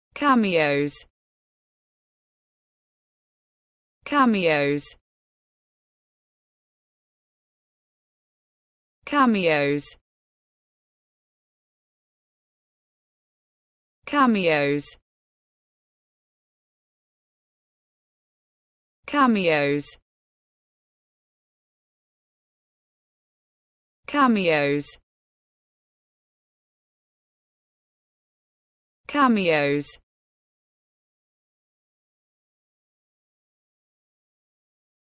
the correct pronunciation of cameos in English.
This video shows you how to pronunciation cameos in English.